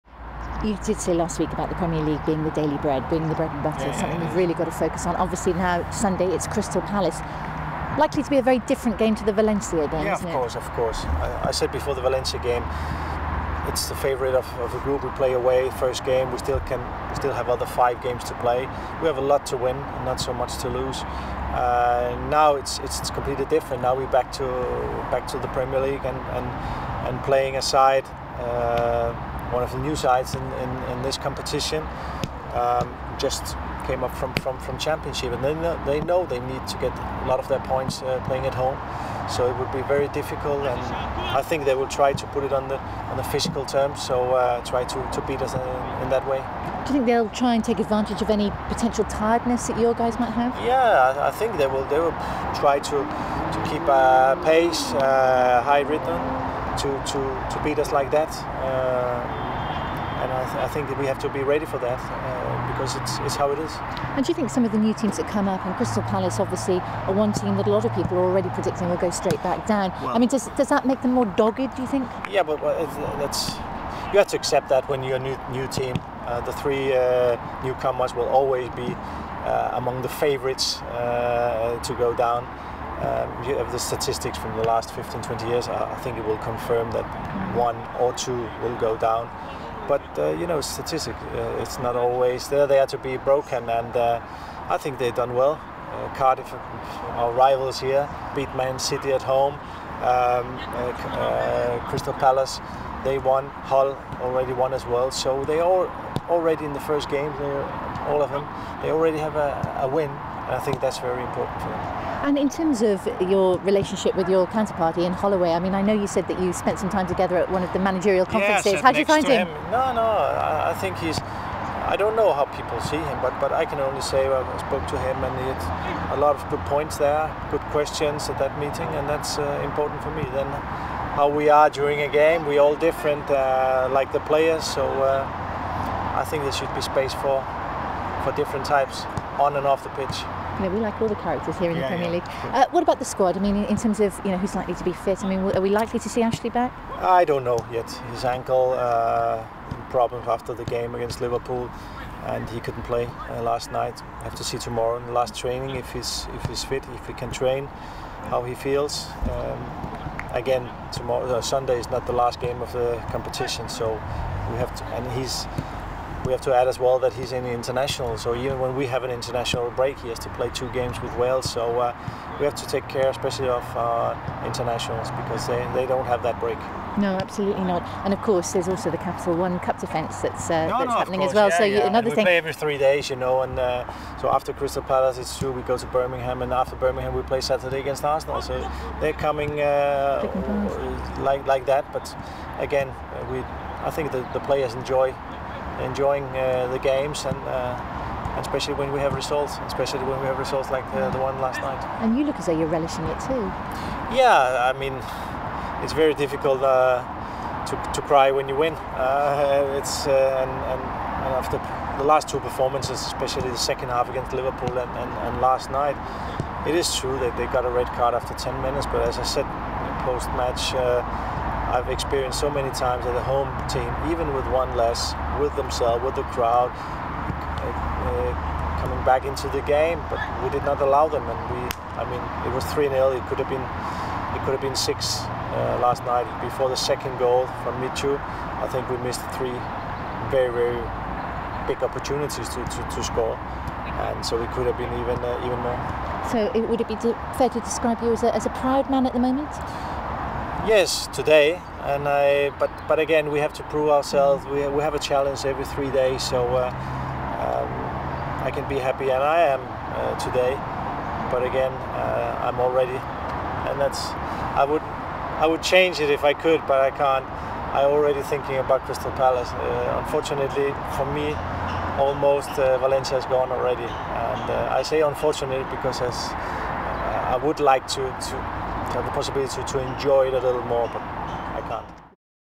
Swansea City Video : Michael Laudrup looks forward to Crystal Palace away
. . . Michael Laudrup looks ahead to the Premier League match with Crystal Palace on Sunday.